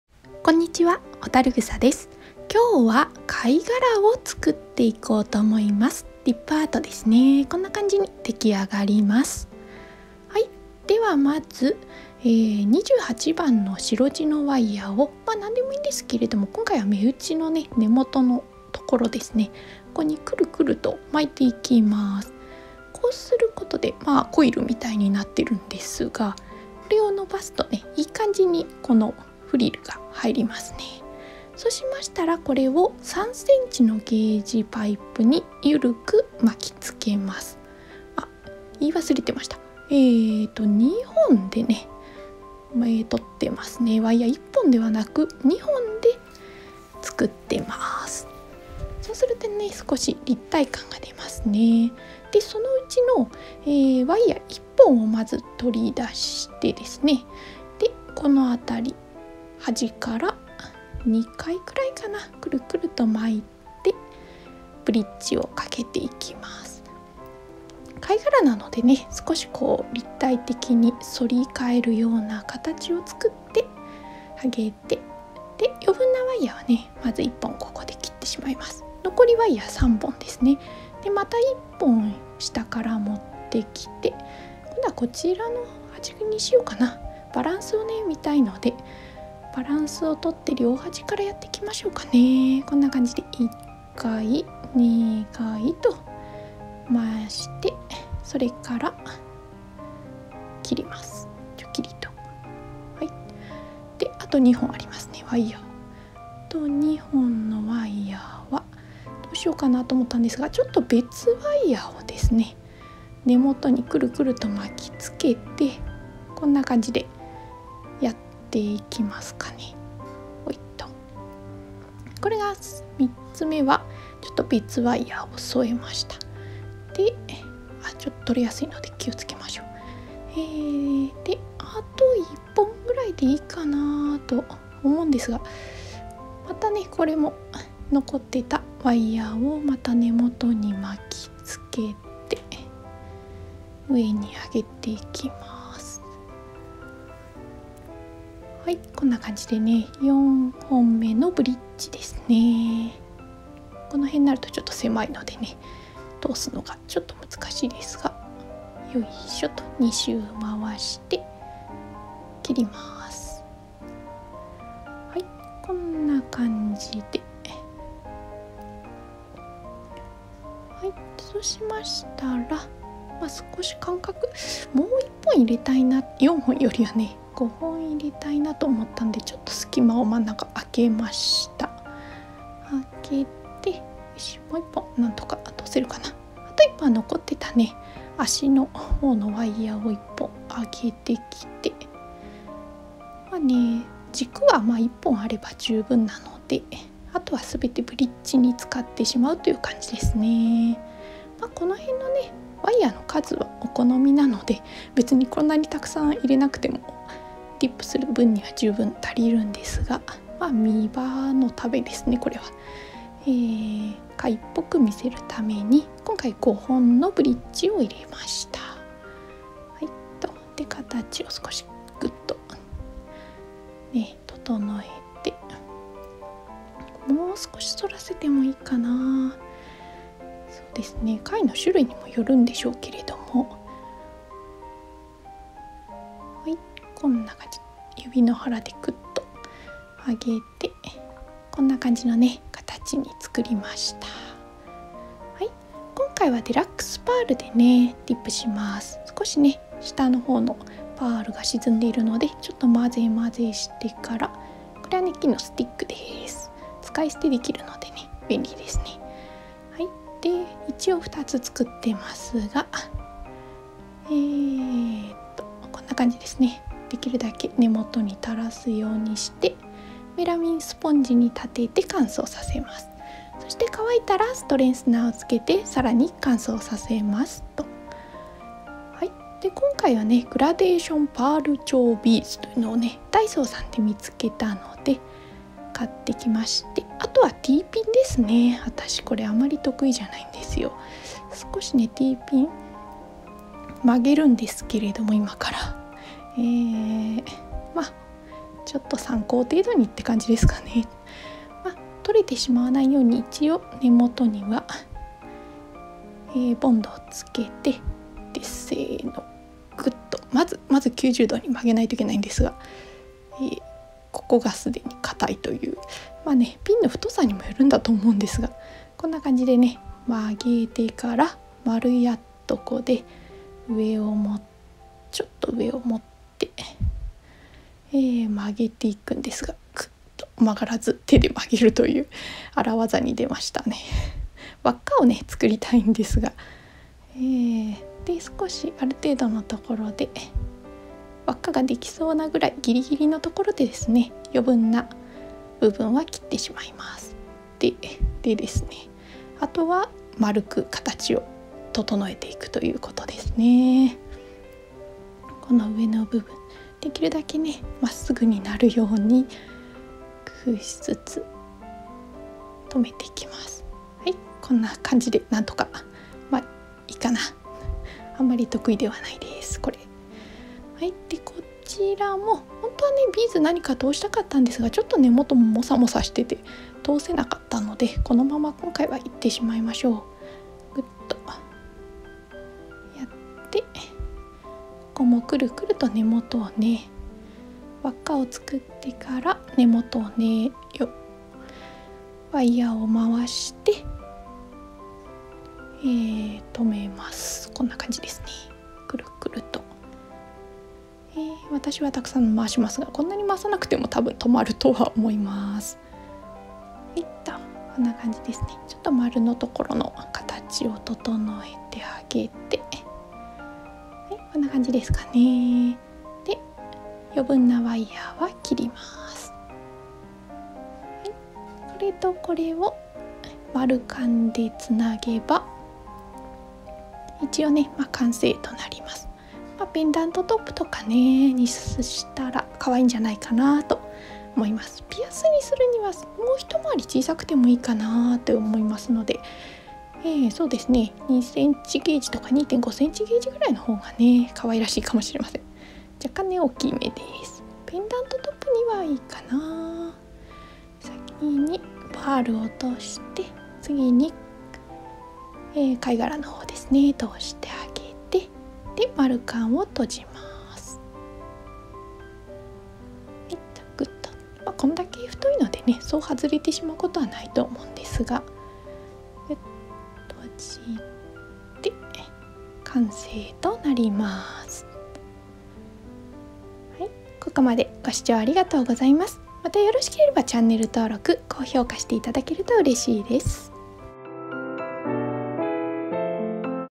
#98 ディップアート 貝殻を作ってみました。ディップアート アメリカンフラワー ハンドメイドです。
趣味でディップフラワー＆アート（アメリカンフラワー）を作っています。
今回はディップアートの貝殻を作ります。
私が作った作品の一部は、メルカリ↓にて購入できます。よろしければご覧ください。

主な材料は↓Amazonにて購入できます(*^_^*)

ディップ液
ディップ液うすめ液
ディップ液の容器
ゲージパイプ
ワイヤー（白地）
ペンチセット
エアブラシ